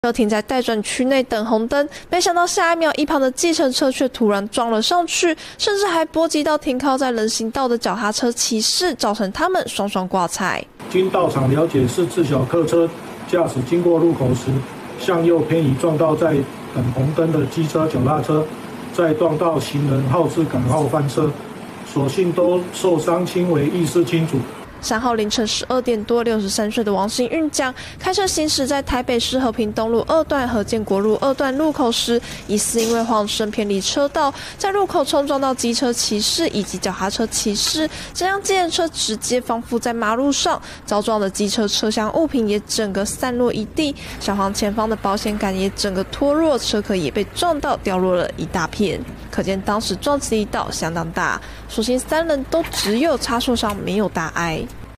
待轉區成「待撞區」？！ 計程車恍神衝撞機車、單車｜鏡新聞
今日（8/3）凌晨12點多，台北大安區和平東路二段與建國南路二段路口發生一起計程車衝撞待轉騎士車禍，甚至還波及到一旁單車騎士，造成3人通通掛彩，詳細情況連線記者劉坤汝。